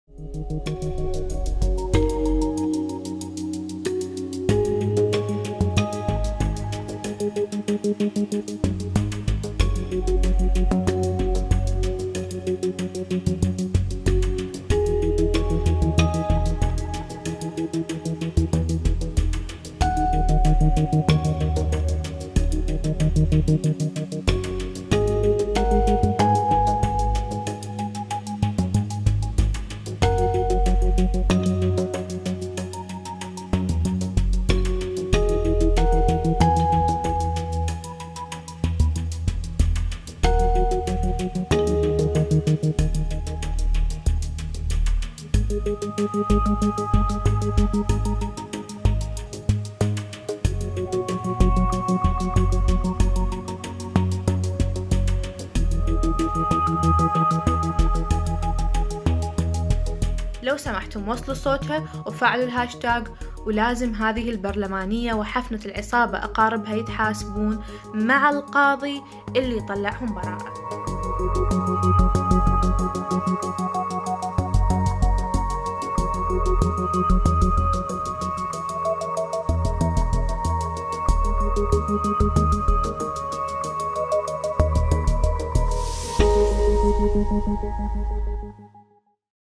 اغتصاب فتاة قاصر يشعل الغضب في العراق
اغتصاب فتاة_الموصل .. واتهام الحشد_الشعبي بارتكاب الجريمة.. وحديث عن إطلاق سراح أحد المتهمين